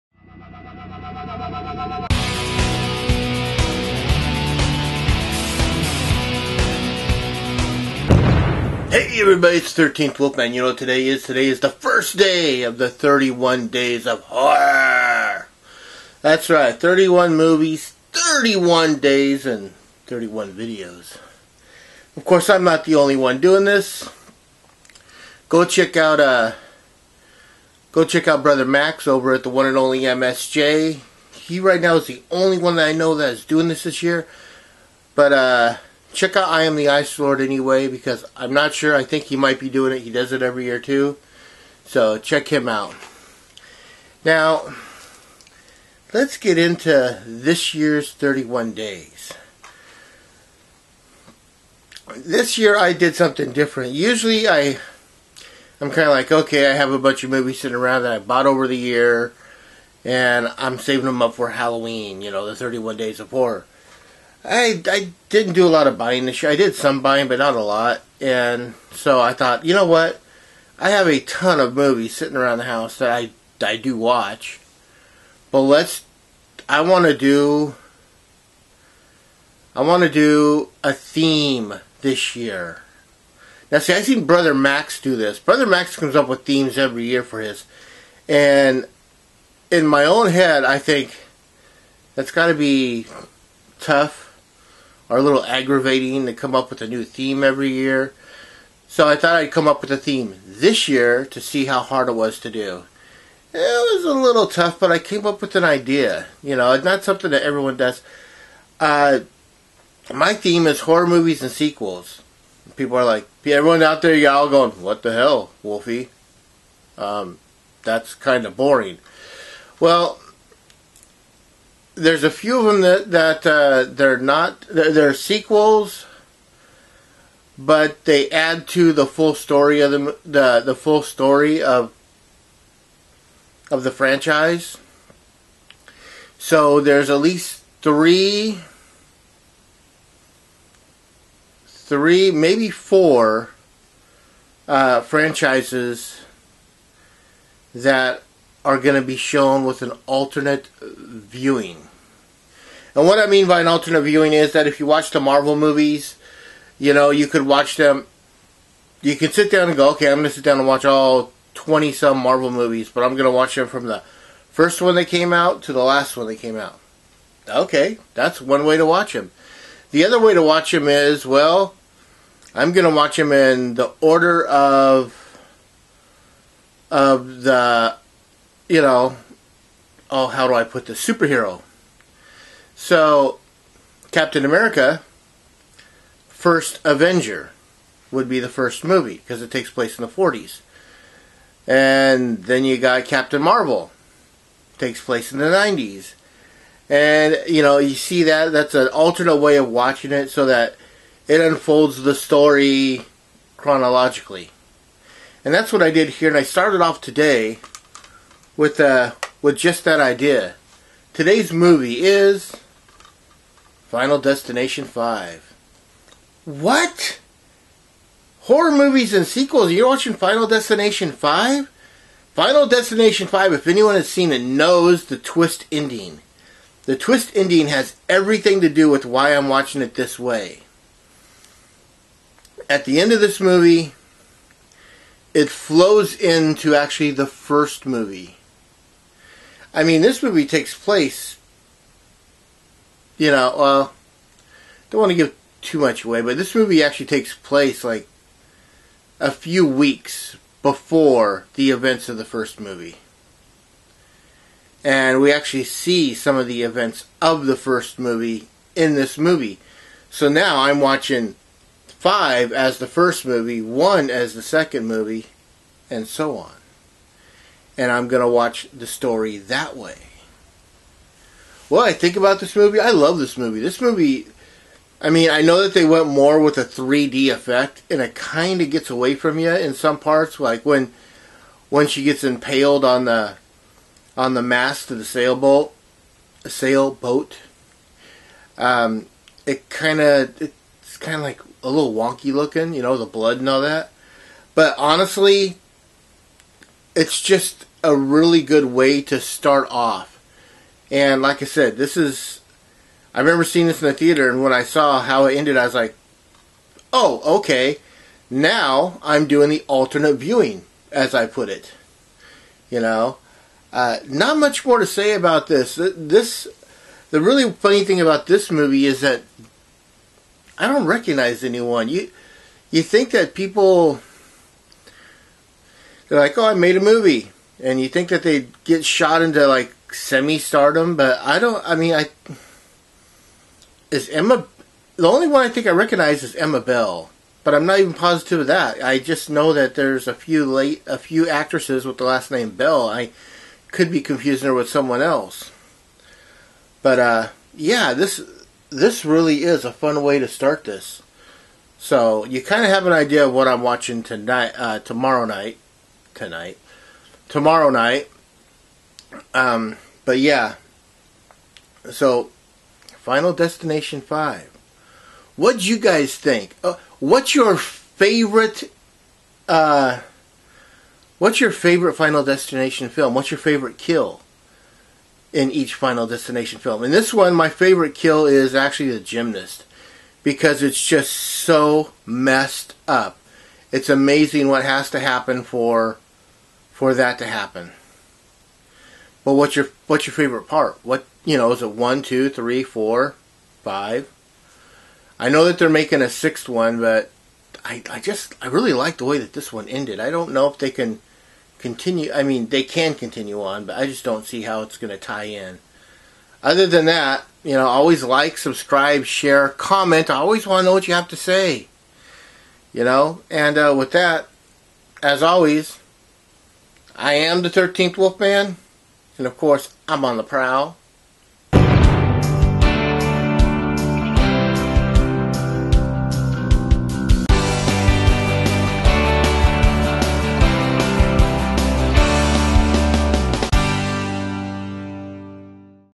31 Days of Horror Day 1 2023 Video
And we are off with this year 31 Days of Horror. I will do a video daily talking about the horror film and, this year I have planned out a theme. Horror movies and their sequels. You will see it is a little different than you think. I hope you enjoy the month.

Schedule
Sunday Mobile Movie Review

Monday: Question of the Week

Tuesday:

Wednesday:

Thursday: Spotlight On

Friday: Reviews